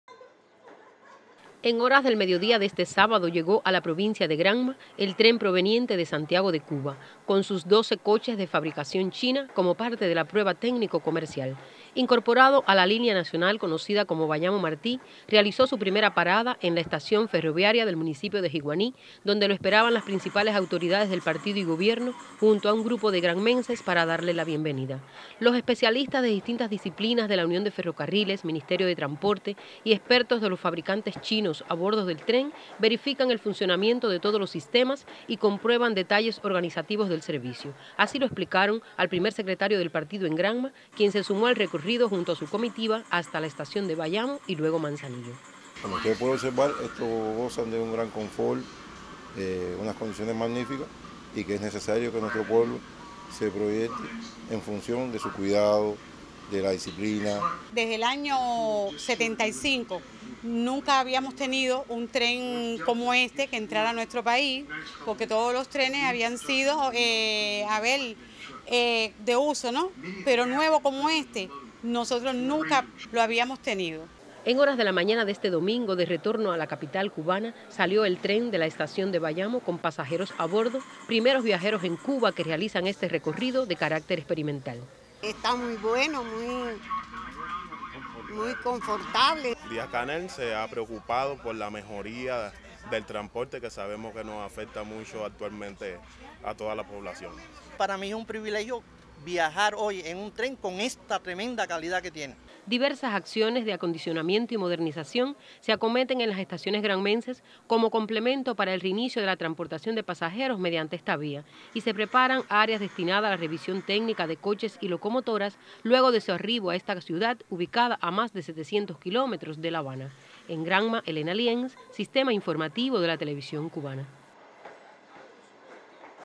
Regresa de Granma a La Habana tren chino en prueba comercial con pasajeros a bordo
En horas del mediodía de este sábado llegó a  la provincia de Granma el tren proveniente de Santiago de Cuba, con sus 12 coches de fabricación china, como parte de la Prueba técnico comercial.
Incorporado a la línea nacional conocida como Bayamo-Martí realizó  su primera parada en la estación ferroviaria del municipio  de Jiguani donde lo esperaban las principales autoridades del Partido y Gobierno junto a un grupo de granmenses para darle la bienvenida.